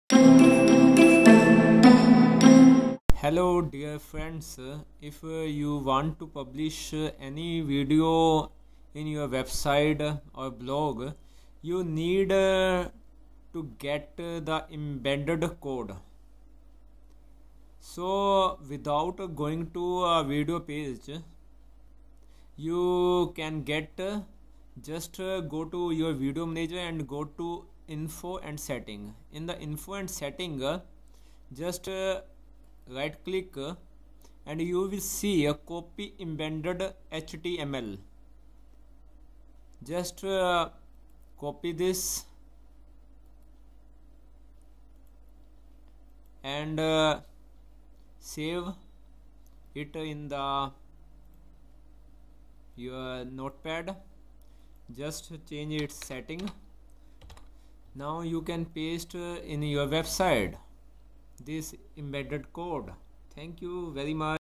How to Get an Embed Code without Fake Views on Youtube
In new theme, youtube team has given very wonderful option for youtube users who upload the videos on daily basis. Now, just go to your video manager, then just go to info and setting. On the video link, press right button of your mouse and your will get embed code of your video. Just copy it and paste in your website. For this, you need not to go to your video page.So, with this, you can save yourself from fake views on youtube.